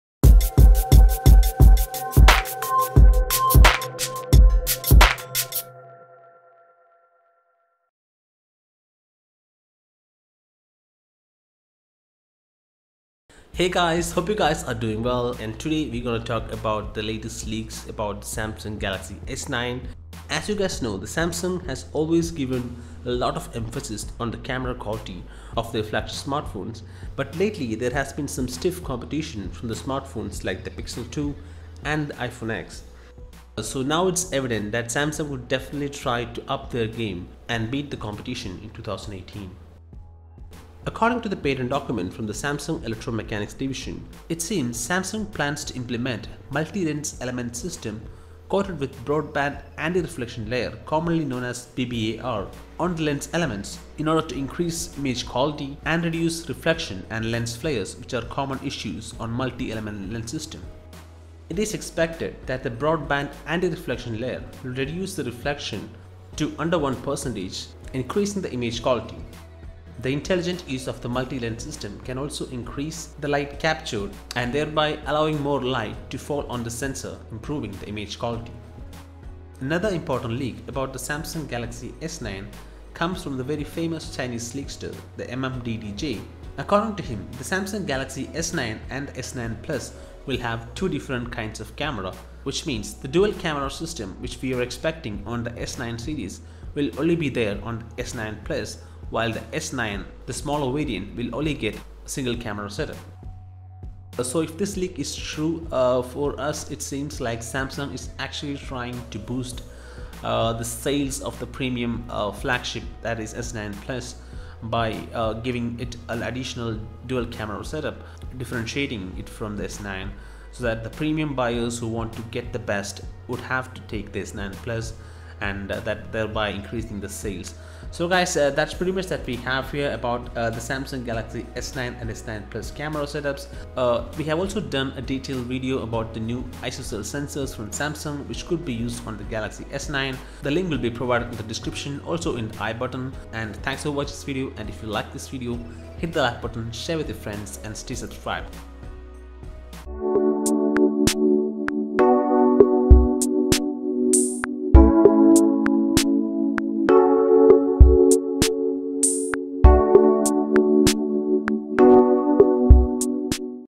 Samsung Galaxy S9 Camera will Adopt High End DSLR Tech,S9 & S9+ will have Different Camera Setup
Samsung smartphone cameras has always been  phenomenal  in terms of image quality especially in the case of its flagship series which always give them an edge over the competition. But now it seems like  the competition is  catching up, the smart phones like Google pixel 2 and iPhone X actually rated much more than the Samsung's own flagships in camera performance.  So now it's evident that Samsung would Definitely try to up their game and beat the competition in 2018.
          According to the patent document from the Samsung electro-mechanics division  it seems Samsung is implementing multi element lens system coated with broad band  anti reflection layer commonly known as BBAR   in order to reduce reflection and lens flares  which are common issues on multi an element  cameras. BBAR is widely used in high end DSLR lense the application of broadband anti reflection layer will reduce reflection to below 1 % and thereby improving the image quality. The multi lens setup can also improve the light captured significantly  which is the key aspect in low light conditions. It can also help to improve slow-motion technology for the S9, which will require a lot more light to be captured to allow for the heavy-duty processing.

All content used is copyright of Techconfigurations, Use/editing of the content(including screenshots) without proper authorization is not allowed.